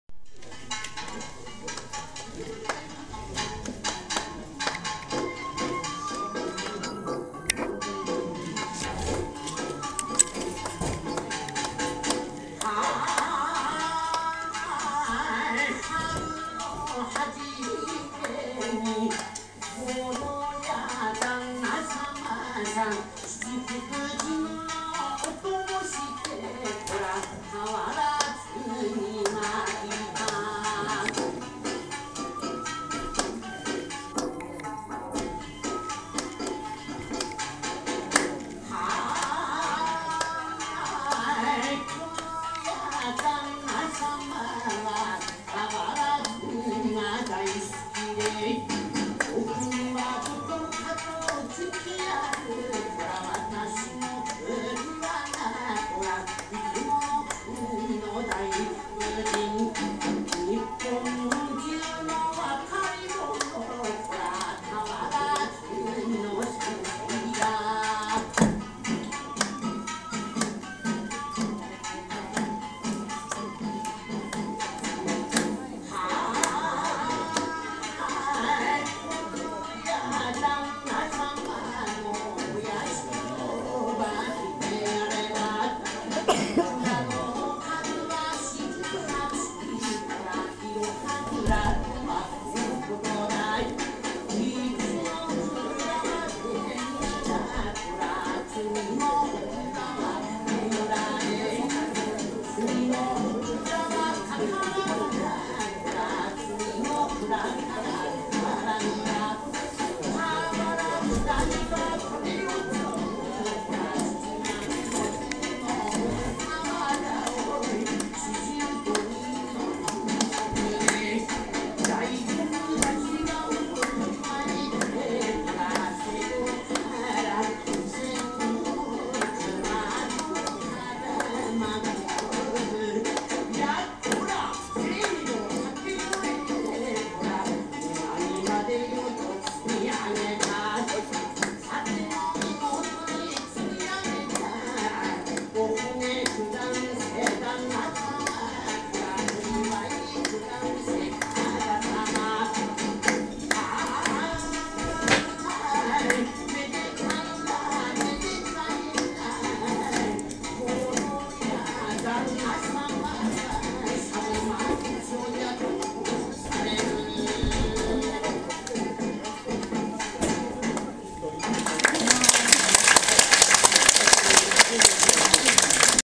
2018.11.13＠曲松＠ふれあい茶話会＠日本舞踊＠その１
平成30年11月13日、曲松連合主催の、ふれあい茶話会。
地域の65歳以上の人、希望者が集まり、日本舞踊の披露。健康・日常生活のお話。お茶とお菓子で歓談。見ず知らずの人も、知り合いもお近づきに、より親しく。
最初は、日本舞踊の団体の自慢の踊り。
お話しは、今回は日常生活の中のヒヤリハットについて。
そのあと、お茶とお菓子で歓談。
最後はみんなで、コーラス。参加者全員の口が開いていました。
参加者59名。